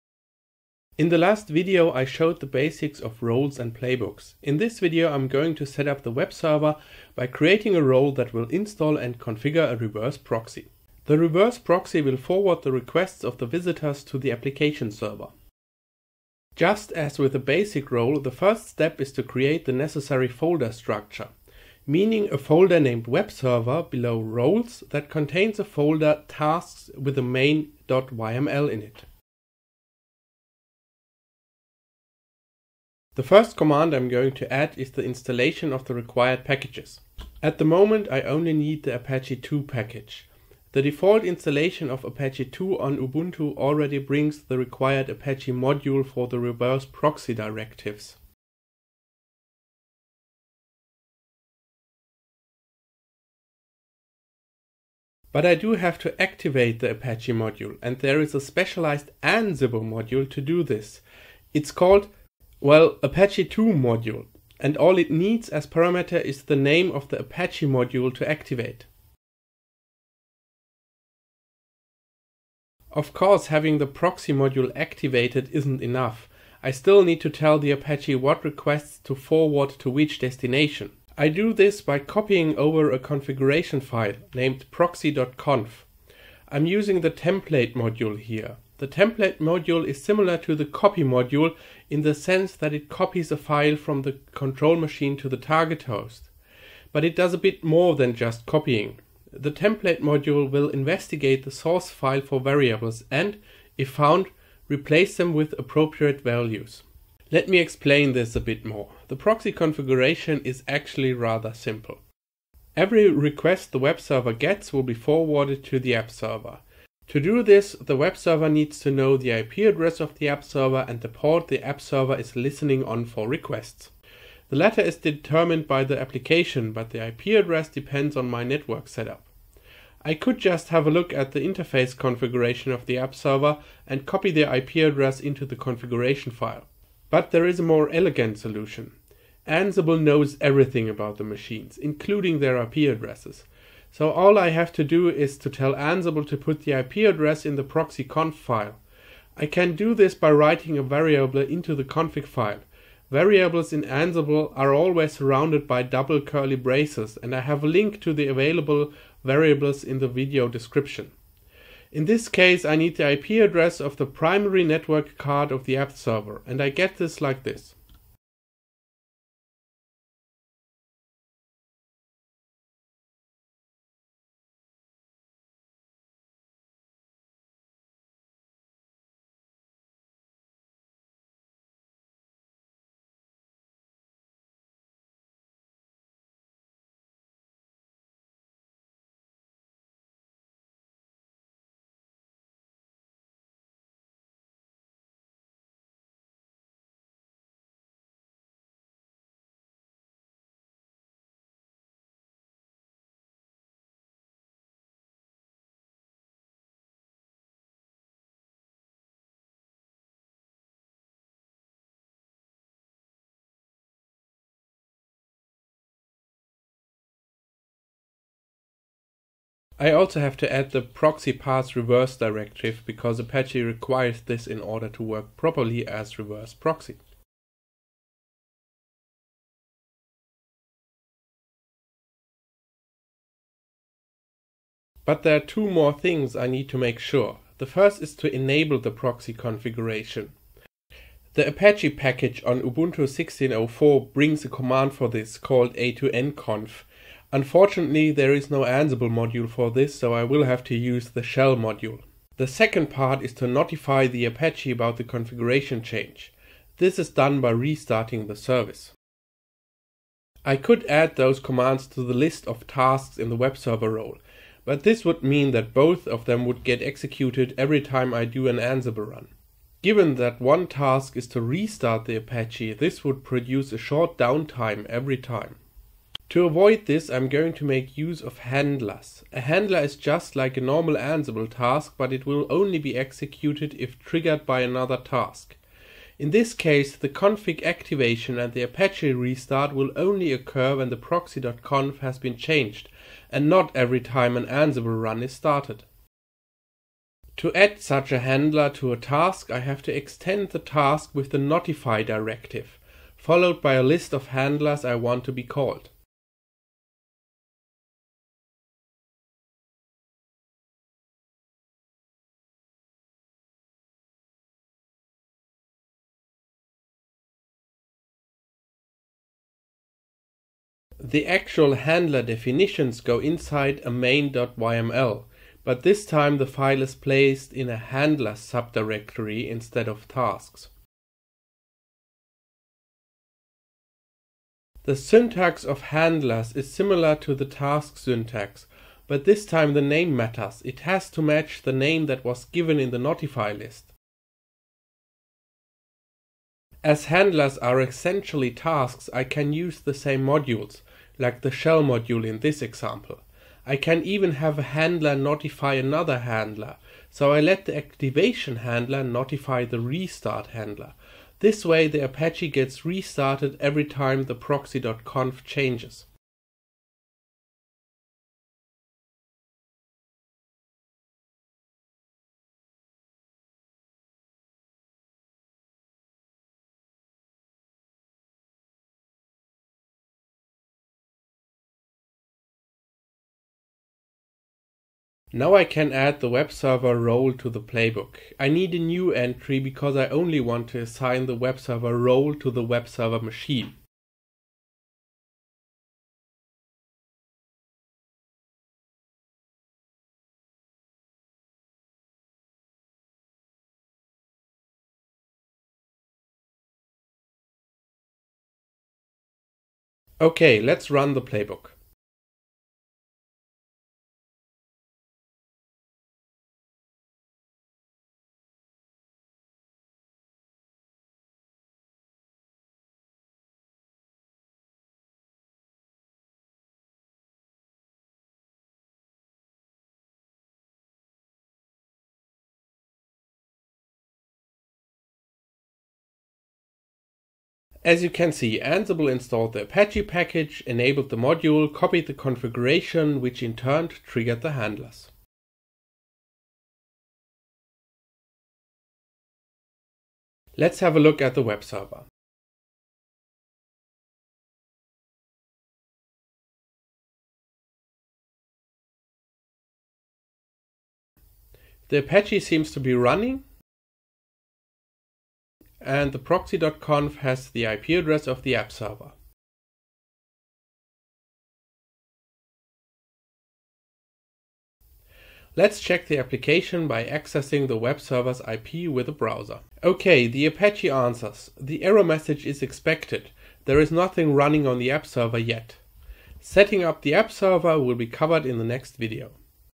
Ansible - A Beginner's Tutorial, Part 3
In this part I'm going to create the role for the webserver which will run a reverse proxy.